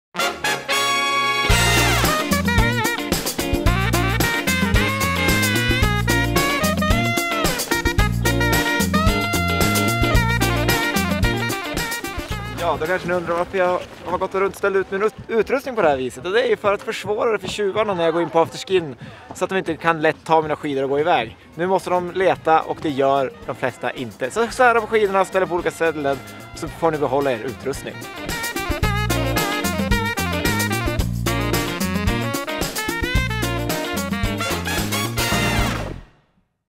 Freeride's Life Hacks # 16 "Lura tjuven"
Skidlivet är svårt. Tom-Oliver Hedvall hjälper dig. Det blir life hacks från den mest basala nivån vidare till riktigt, riktigt avancerade tips. Del sexton: Lura tjuven.